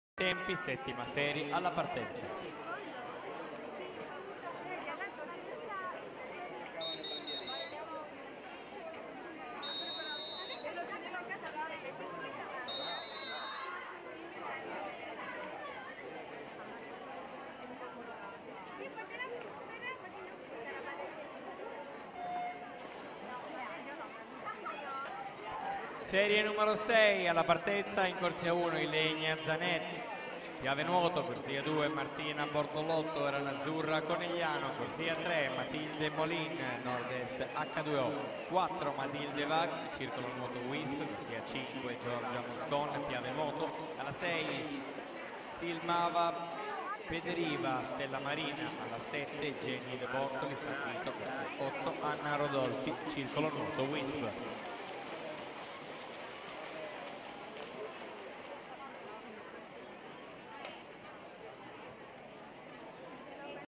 50 Dorso Ass. Femminile Agoniste (Serie 7) - 5 Treviso Swim Cup
Atleti partecipanti:
in corsia 1 URSACHI ROBERTA CARINA (2003) della soc. SAN VITO iscritta con 00:34.57
in corsia 2 DE FAVERI ELISA (2004) della soc. PIAVE NUOTO iscritta con 00:34.47
in corsia 3 PANZAVOLTA SARA (2002) della soc. RINASCITA TEAM ROMAGNA iscritta con 00:34.35
in corsia 4 SCAPOLI MARTA (2004) della soc. AMATORI NUOTO iscritta con 00:34.22
in corsia 5 TESEI ALESSIA (2002) della soc. RINASCITA TEAM ROMAGNA iscritta con 00:34.22
in corsia 6 BETTARELLO GIULIA (2004) della soc. PIAVE NUOTO iscritta con 00:34.37
in corsia 7 CORDA MARTINA (2004) della soc. HYDROS iscritta con 00:34.55
in corsia 8 MEROTTO GIORGIA (2005) della soc. ZEUS LAB MONTEBELLUNA iscritta con 00:34.92